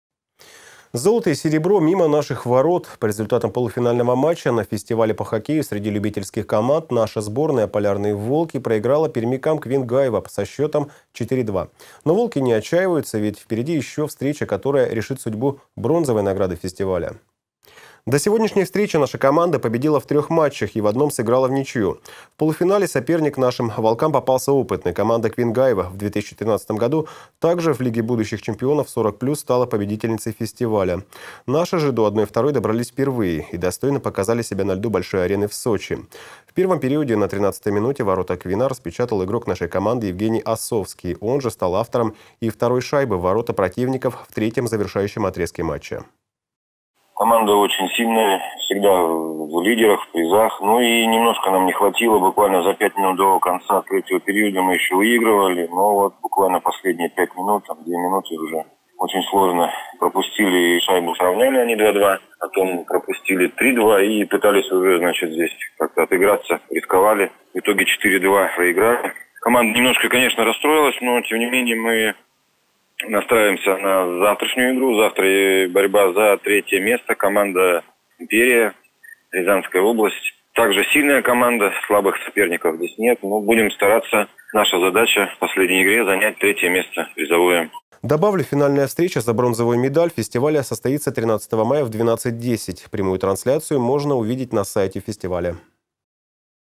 «Полярные волки» из НАО поборются за бронзу на фестивале по хоккею в Сочи
Золото и серебро мимо наших ворот. По результатам полуфинального матча на фестивале по хоккею среди любительских команд сборная Ненецкого округа «Полярные волки» проиграла пермякам «Квин-Гайва» со счетом 4:2. Но «волки» не отчаиваются, ведь впереди еще встреча, которая решит судьбу бронзовой награды фестиваля.

До этой встречи наша команда победила в трех матчах и в одном сыграла вничью. В полуфинале соперник нашим «волкам» попался опытный, команда «Квин-Гайва» в 2013 году также в лиге будущих чемпионов 40+ стала победительницей фестиваля. Наши же до одной второй добрались впервые и достойно показали себя на льду Большой арены в Сочи. В первом периоде на 13-й минуте ворота «Квина» распечатал игрок нашей команды Евгений Осовской. Он же стал автором и второй шайбы в ворота противников в третьем завершающем отрезке матча.